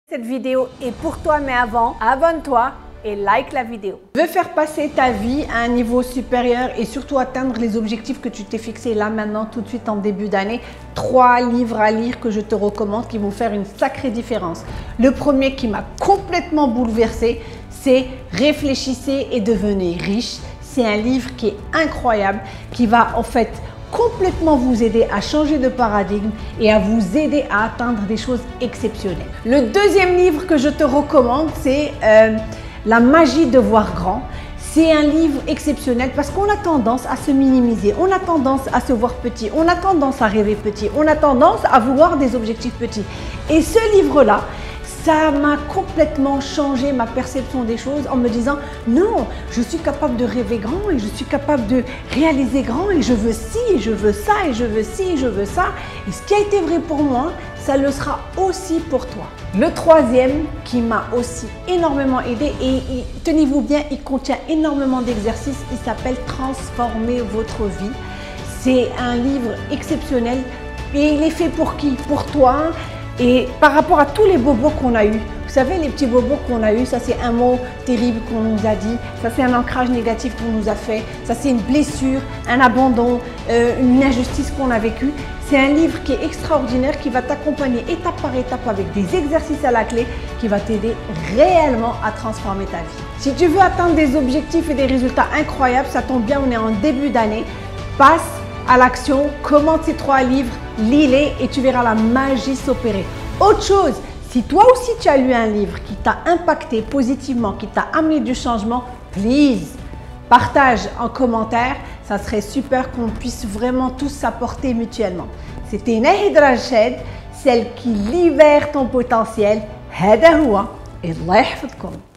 3 livres à lire cette année pour décoller 🔥
Avez-vous déjà senti que vous pourriez atteindre plus dans la vie? Il est temps d’agir! Voici trois livres qui vous aideront à débloquer votre potentiel et à passer à un niveau supérieur 🔥
partagez avec moi en commentaires vos retours ⬇️

1.) 3 Minutes Pour Changer De Vie
2.) Motivation, Confiance En Soi & Leadership